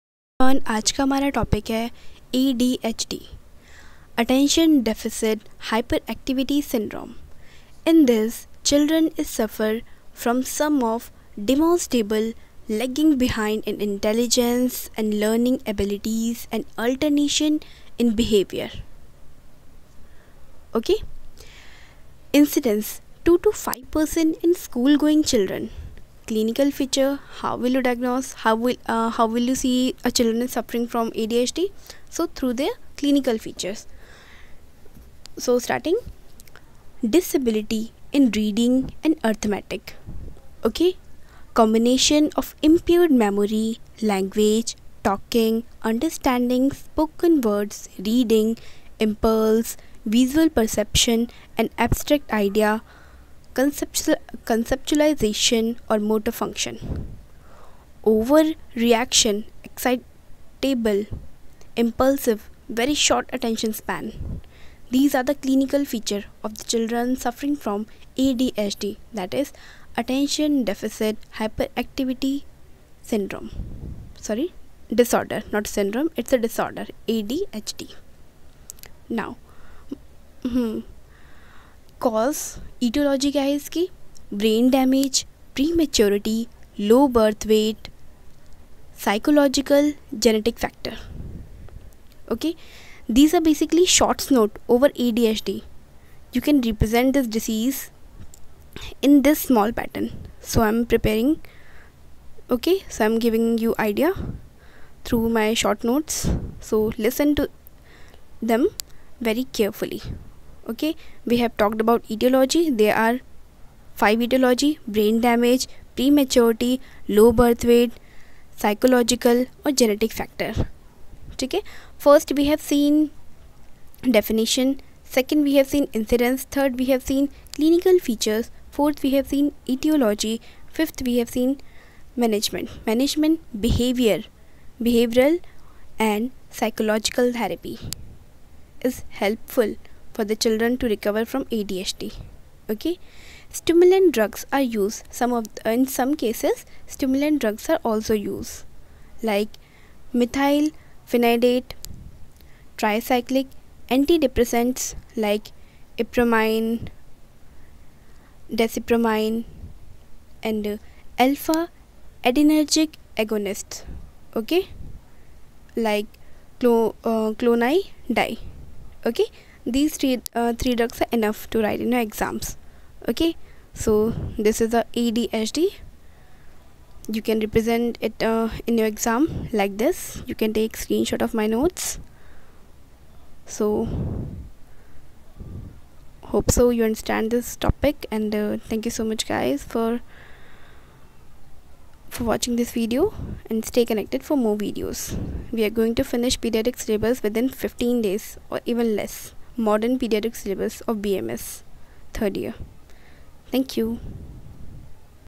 ADHD pediatric bams
ADHD - Attention deficit hyperactivity disease short notes for exam purpose 😇